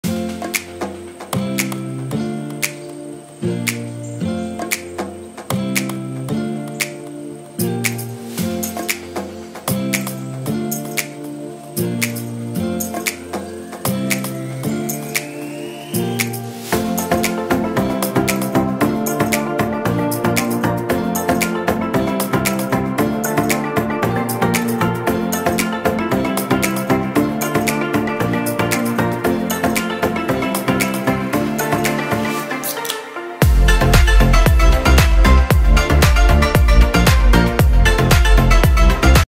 How to use ZS-VTMP40 Magnetic Pump Liquid Filling Machine (4 Nozzles)
Features:
1. Multiple filling nozzles can be customized as production need.
2. Can work with automatic capping and labeling machine to realize automated production.